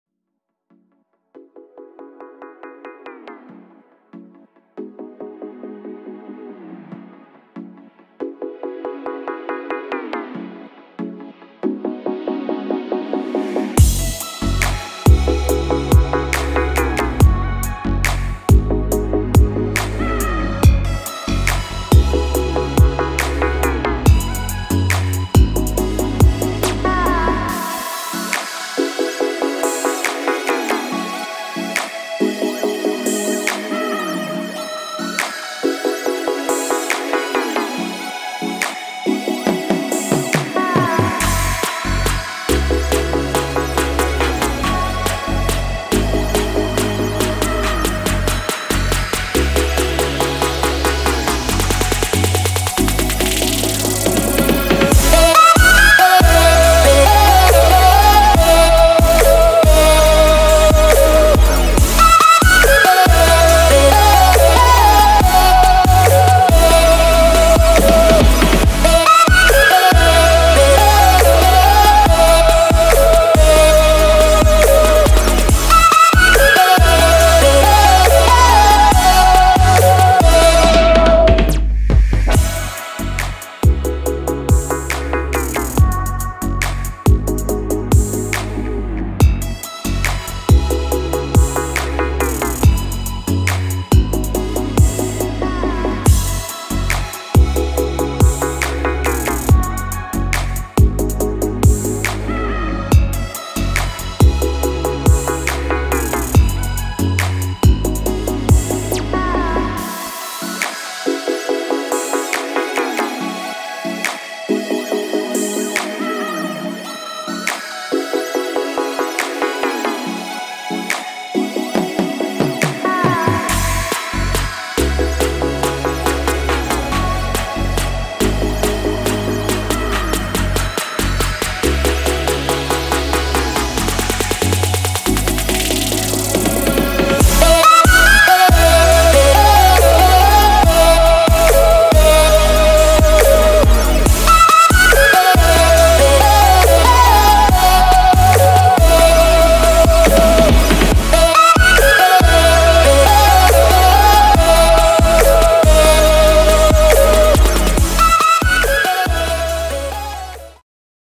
Glasgow Scotland drone footage
a flight along the clyde on a sunny day